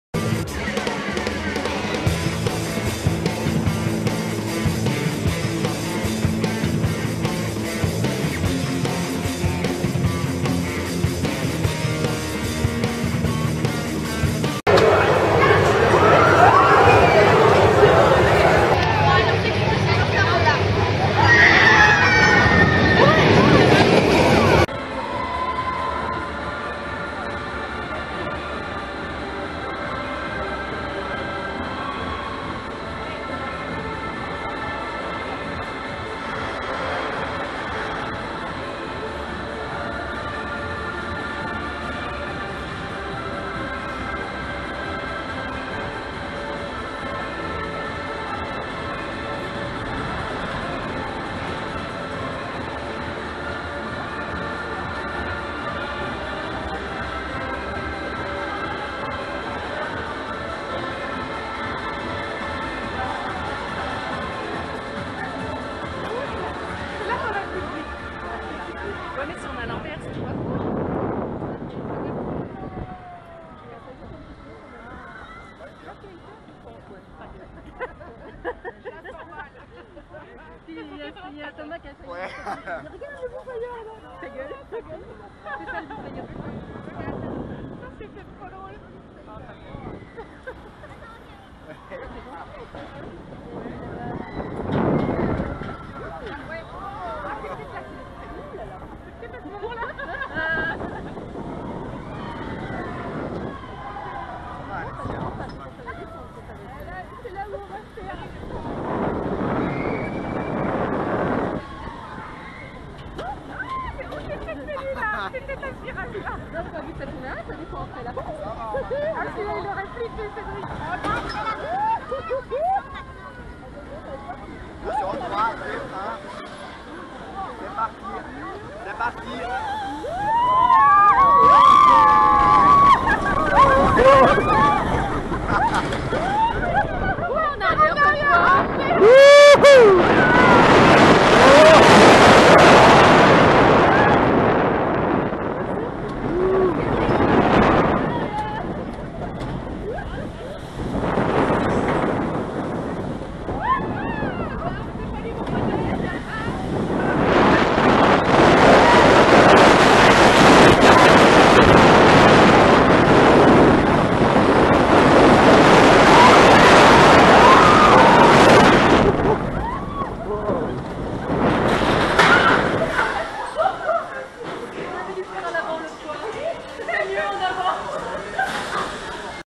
Euromir Roller Coaster Europa Park, Germany
Jump aboard Euromir the Russian space themed roller coaster at Europa Park Germany